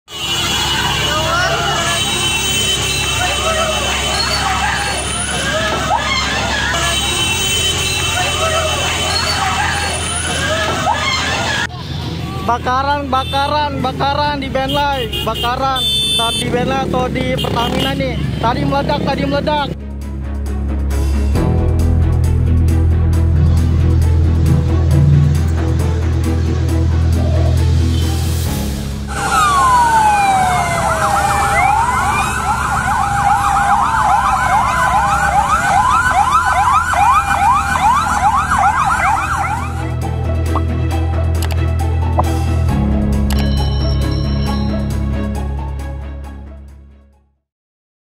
[BREAKING NEWS] Pipa Bensin Pertamina di Plumpang, Jakarta Utara, Terbakar
Kebakaran yang terjadi di Depo Pertamina Plumpang, Kelapa Gading, Jakarta Utara, Jumat (3/3), merembet ke rumah warga sekitar. Api menjalar hingga membakar sejumlah rumah yang tak jauh dari Depo Pertamina. Selengkapnya simak video ini.